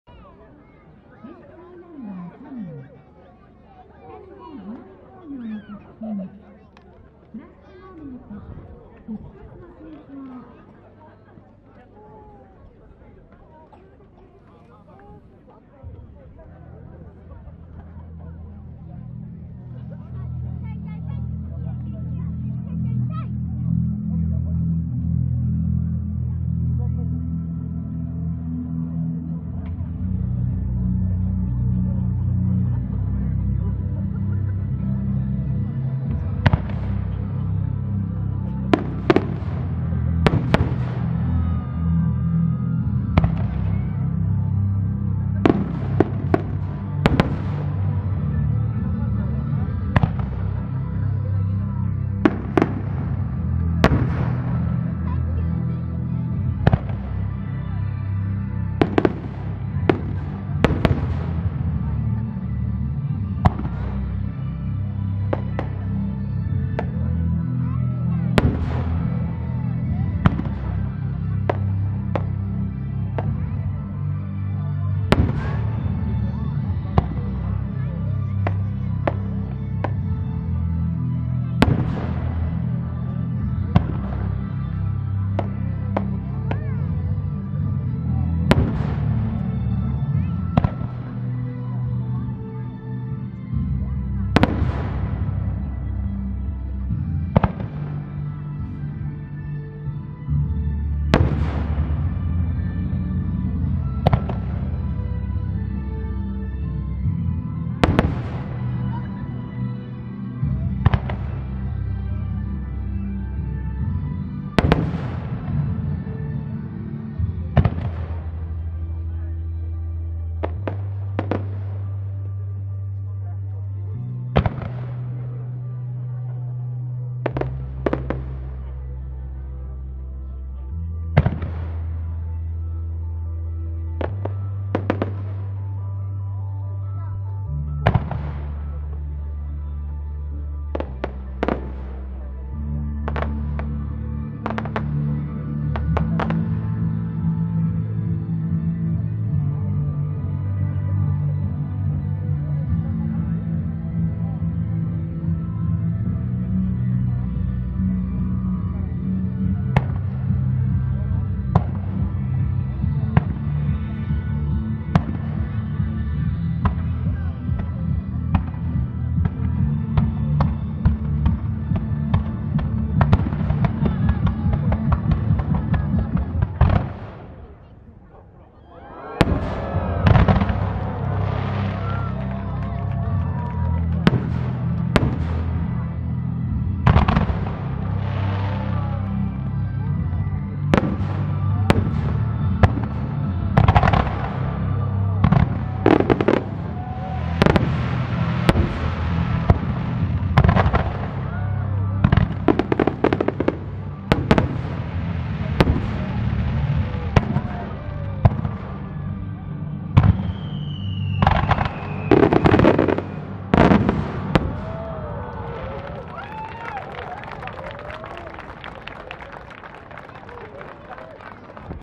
能代の花火「FRASH MOMENT ～復活の閃光～」北日本花火興業 Noshiro Fireworks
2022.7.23 第18回港まつり　能代の花火「能代にはある、能代にしかない、復活のノロシ」
 (プログラム) オープニングファイアー　「FRASH MOMENT ～復活の閃光～」
担当：北日本花火興業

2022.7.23 The 18th Minato Festival Noshiro fireworks "There is in Noshiro, only in Noshiro, Noroshi of resurrection"
(Program) FRASH MOMENT ~Flashes of Resurrection~